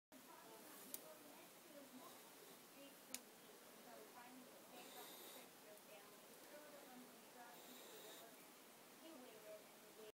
Random string on towel
Found random string on a towel and was messing around